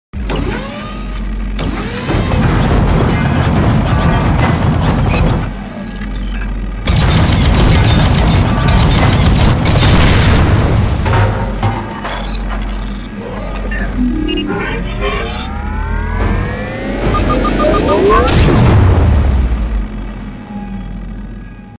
Blowing up the House for New Years
This is the free effect from FX Guru for the next 24 hours. Just grabbed it to test out.